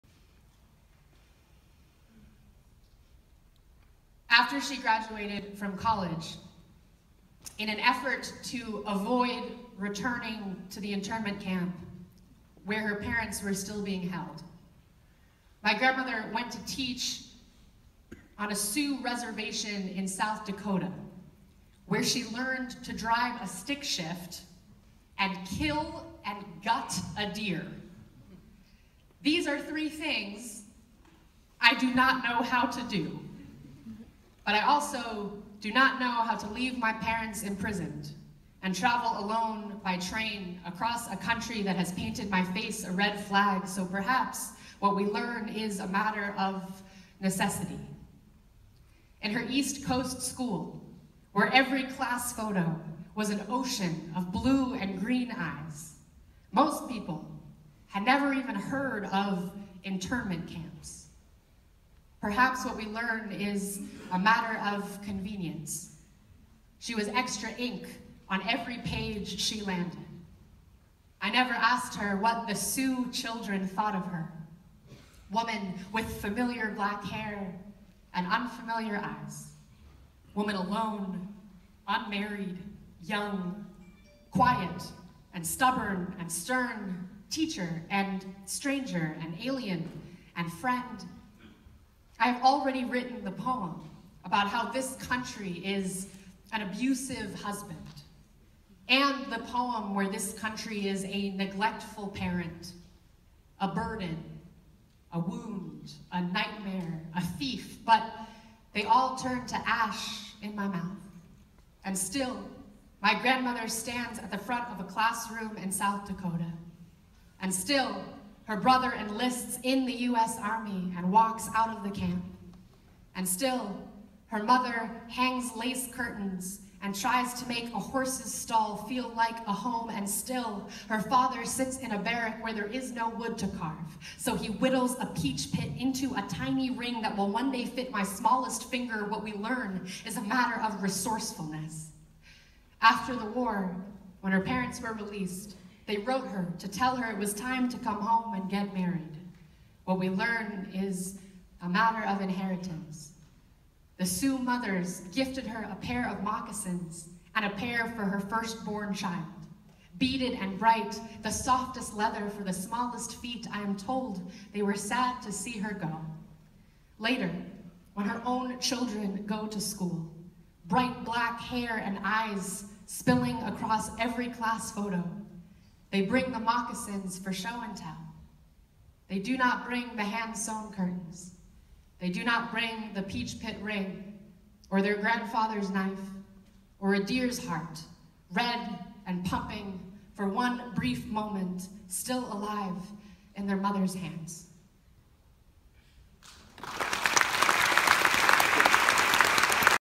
Sarah Kay at UC Irvine - Poem about Japanese-American Internment Camps
Sarah Kay at UC Irvine - Poem about Japanese-American Internment Camps she wrote in response to Trump era talk of reviving this heinous idea.